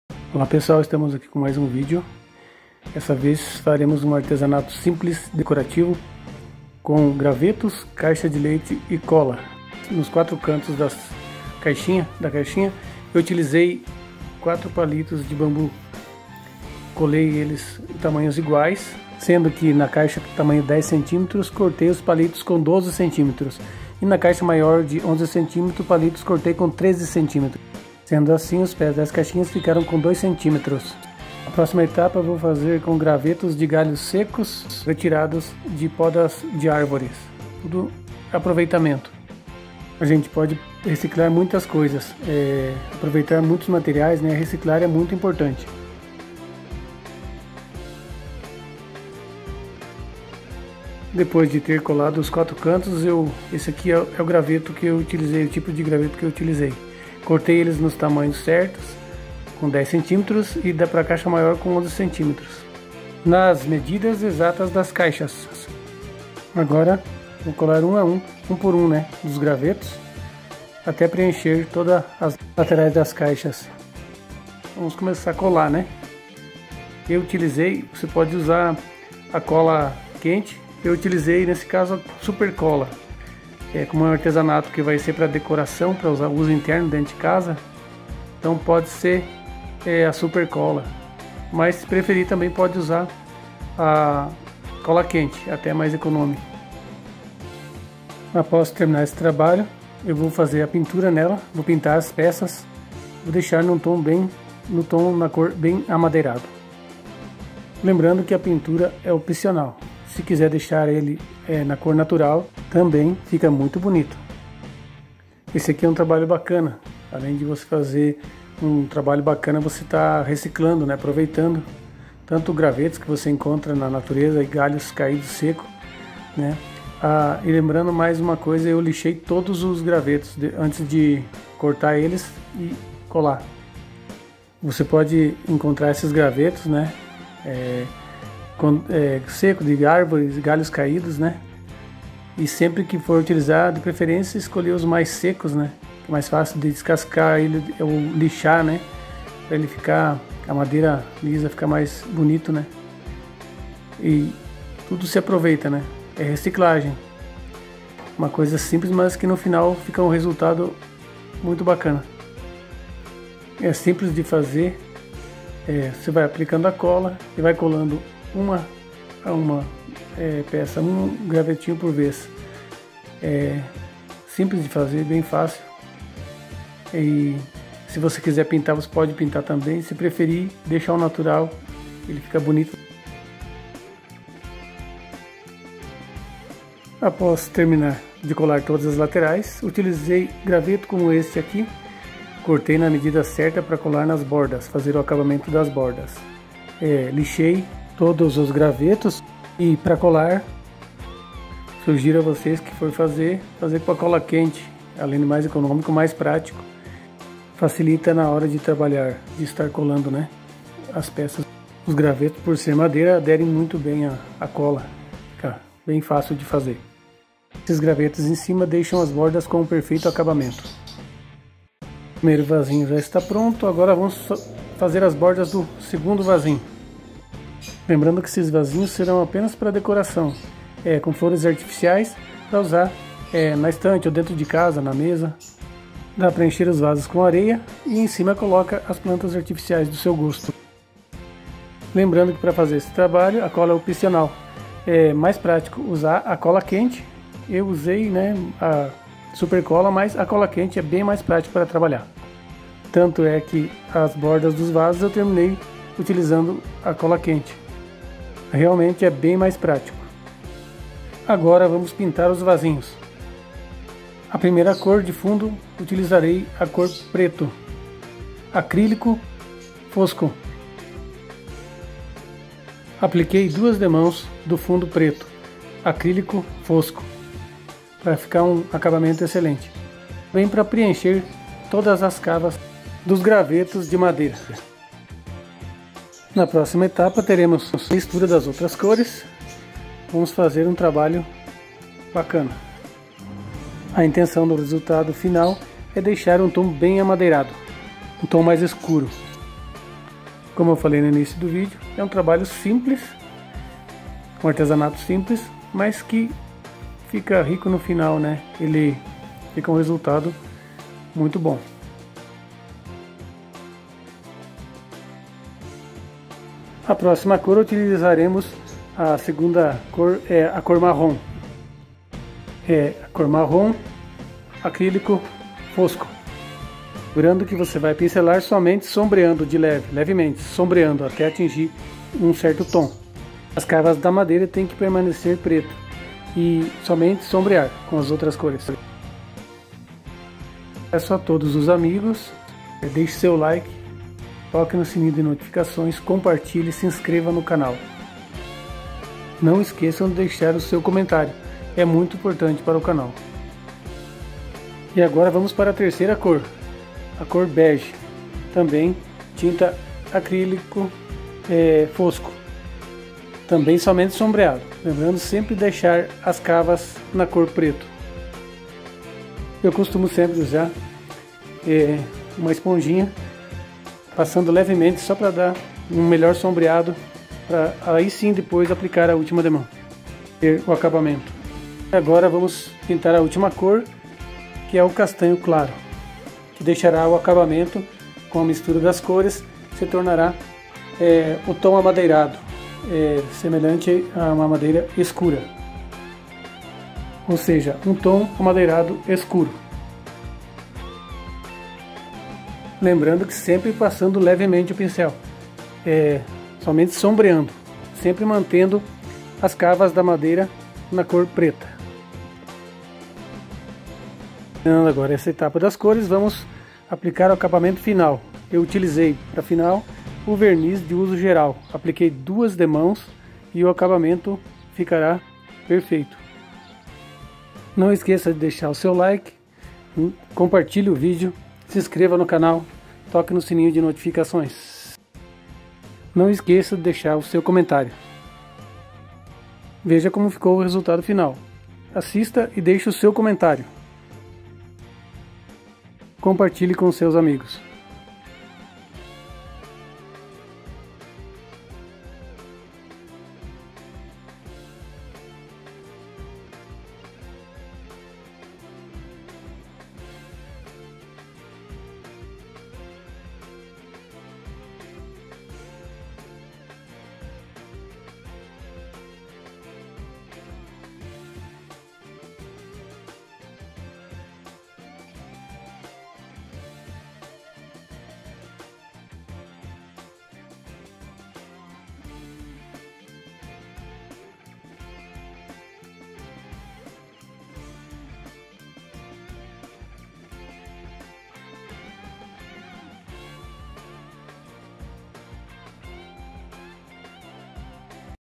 IDÉIA COM GRAVETOS - Diy Pintura Ton Escuro SOBRE CAIXAS DE LEITE
Músico: Michael Ramir C.

VASOS CACHEPÔS RECICLADOS
FEITOS COM GRAVETOS E CAIXAS DE LEITE

MATERIAIS USADOS
2 CAIXAS DE LEITE

GRAVETOS DE MADEIRA

COLA QUENTE NAS BORDAS

SUPER COLA NAS LATERAIS

BAMBU NAS CANTONEIRAS ( PÉS) DOS VASOS

FUNDO COR : PRETO ACRÍLICO FOSCO

SEGUNDA COR  :MARROM

TERCEIRA COR  : BEGE

QUARTA COR : CASTANHO CLARO

ACABAMENTO FINAL : VERNIZ DE USO GERAL

TOQUE NO SININHO DE NOTIFICAÇÕES 🔔